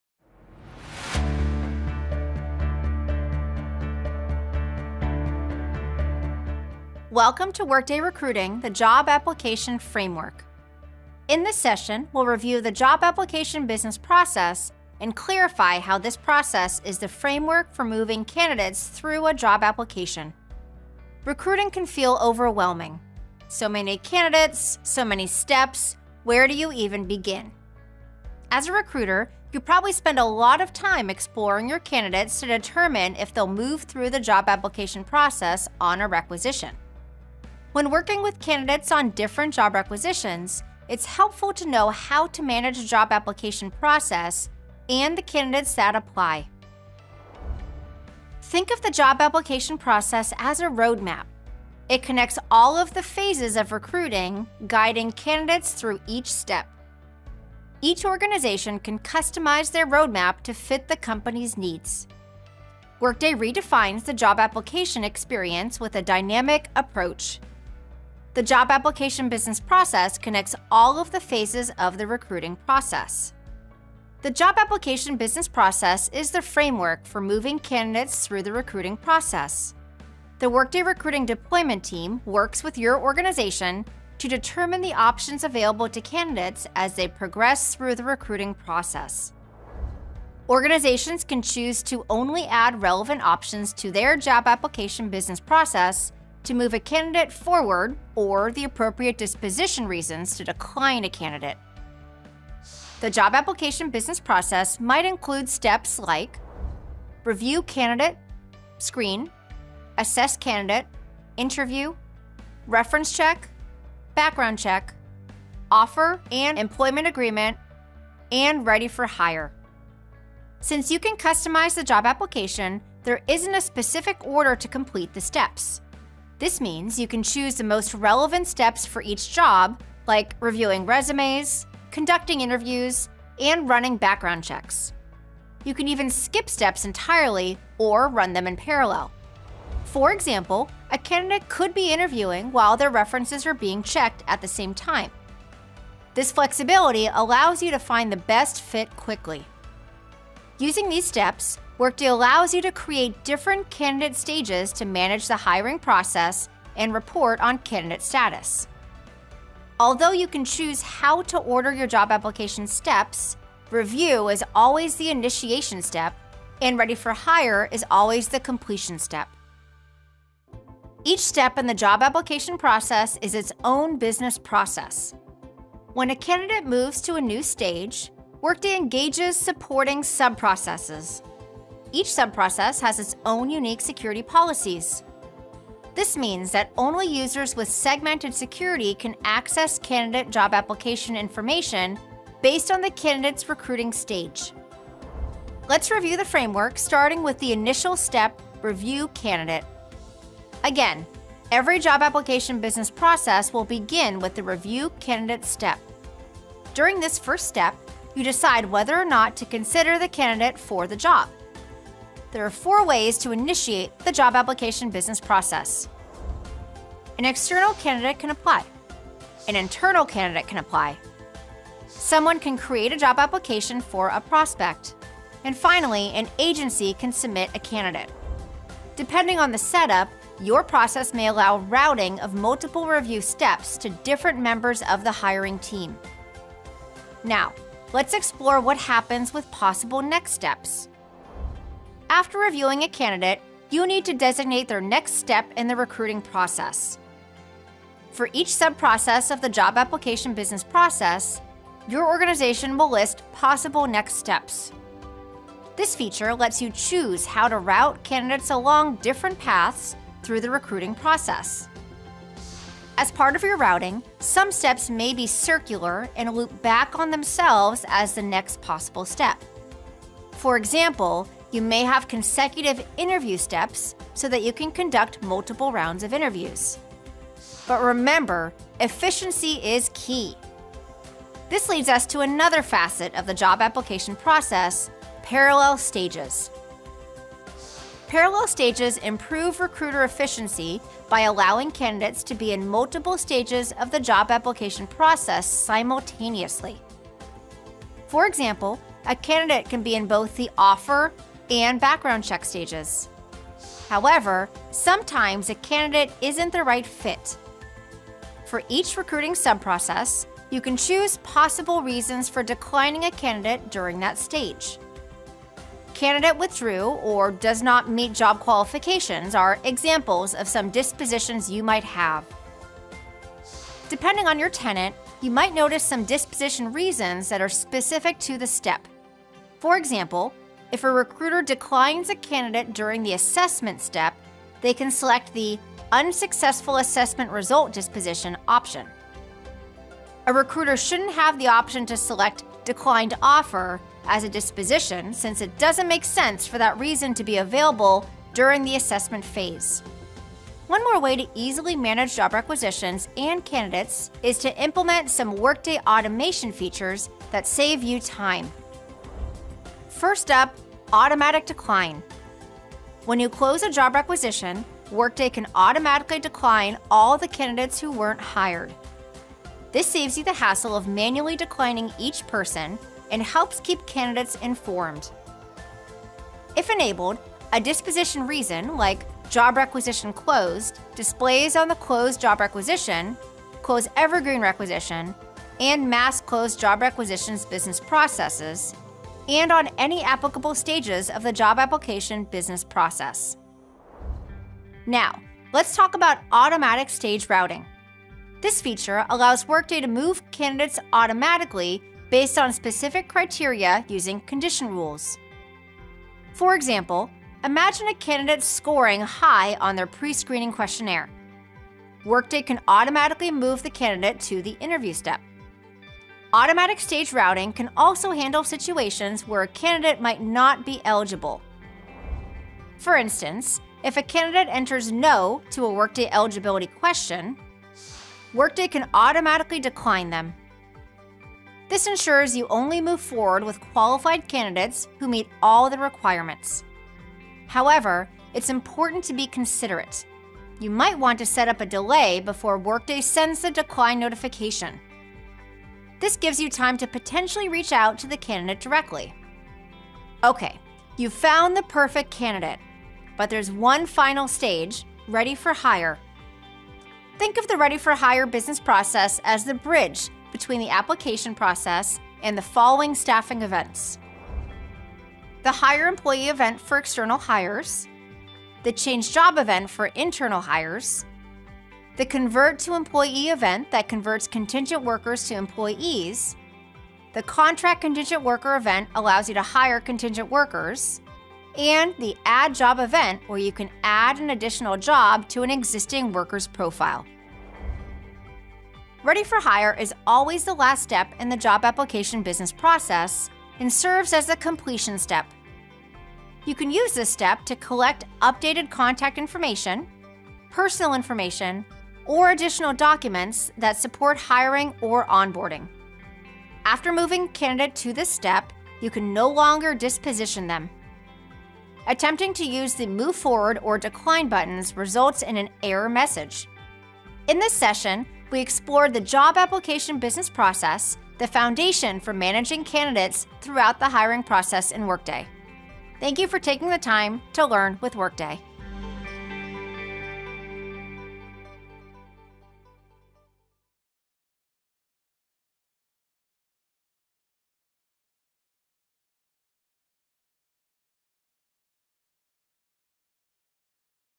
How to Navigate the Job Application Business Process | Workday Recruiting
Learn about the Job Application business process and how it manages the movement of candidates during their application.